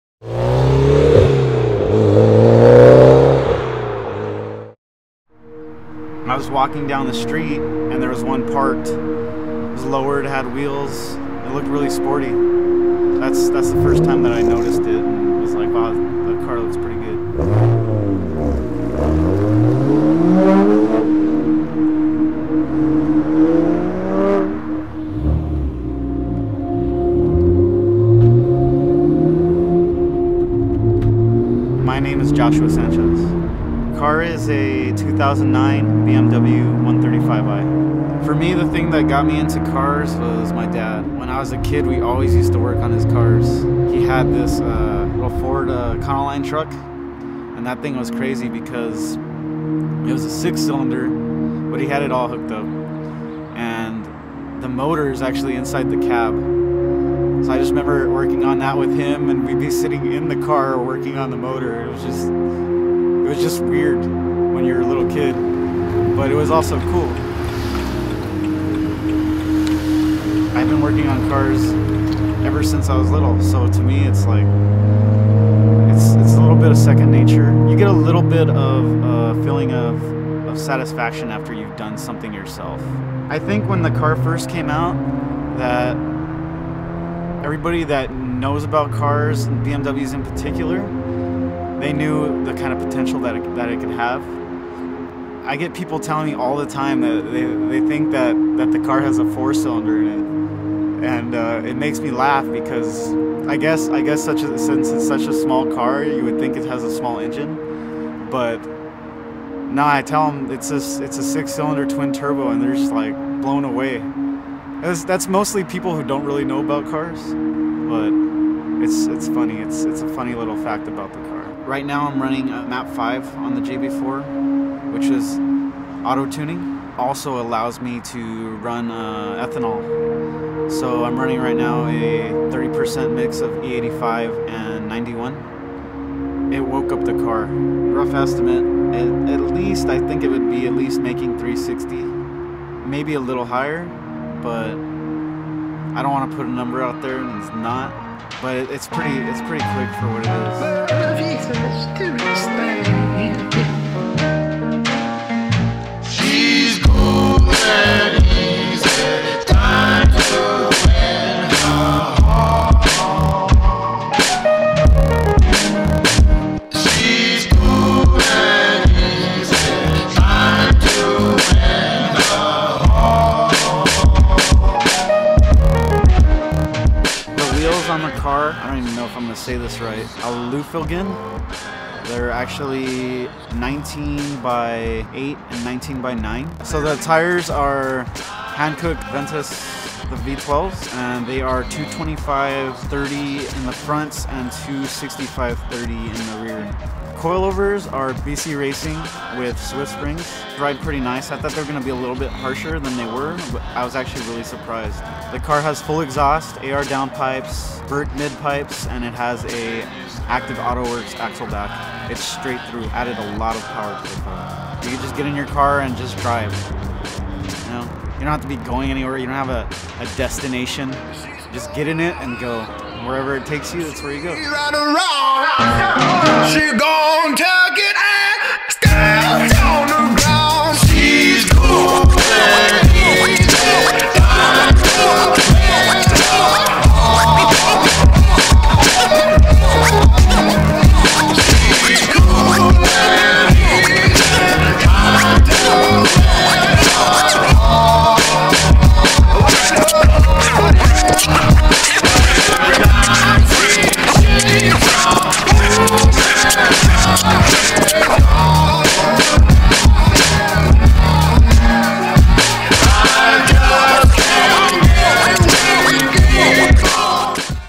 [SHORT FILM] BMW 135i TWIN TURBO
A mini-documentary about Josh Sanchez and his BMW 135i

Editor - Alex Shaw
Director of Photography - Alex Shaw
Producer -Alex Shaw
Producer Assistant - Alex Shaw